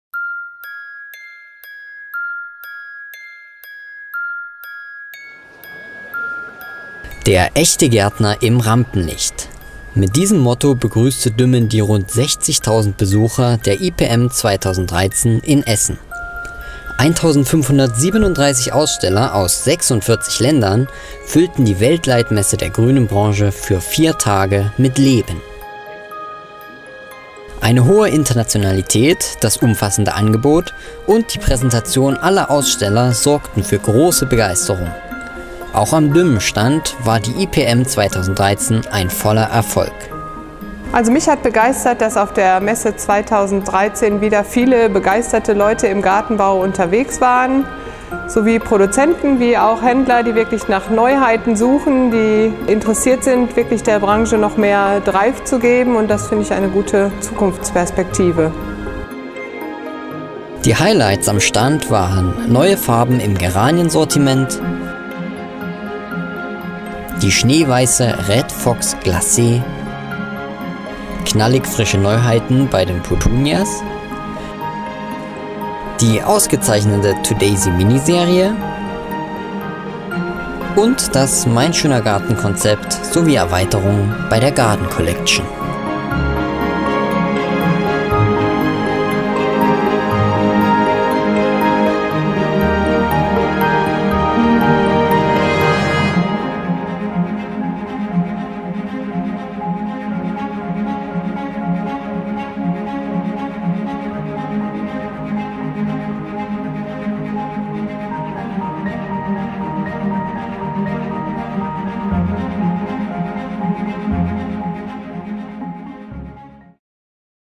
Intro IPM Film_Deutsch
Erleben Sie einen kleinen Rückblick auf die IPM 2013. Zum Ende des Videos kann zu beliebigen Themen weiter navigiert werden.